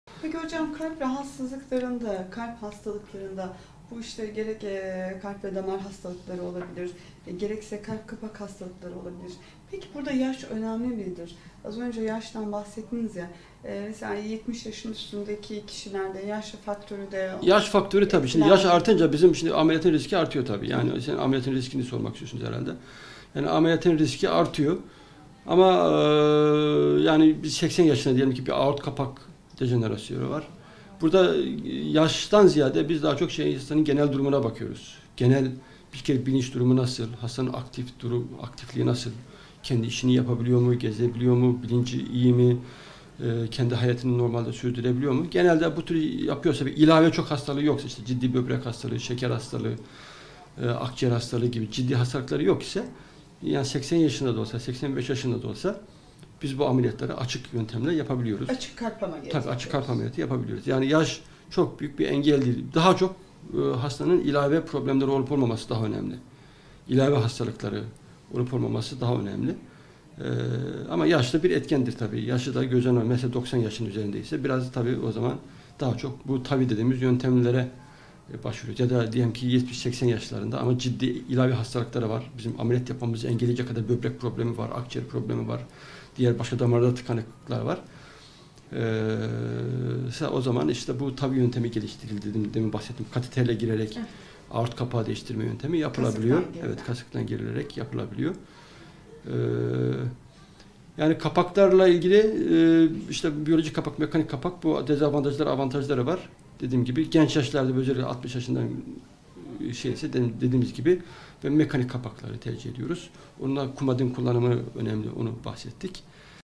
Kalp Kapak Hastalıklarında Yaş Faktörü Nedir ?
Avrasya Hastanesi Kalp Ve Damar Cerrahisi Uzm. Dr. Ali Rıza Cenal '' Kalp Kapak Hastalıklarında Yaş Faktörü Nedir ? '' hakkında detayı bilgiler veriyor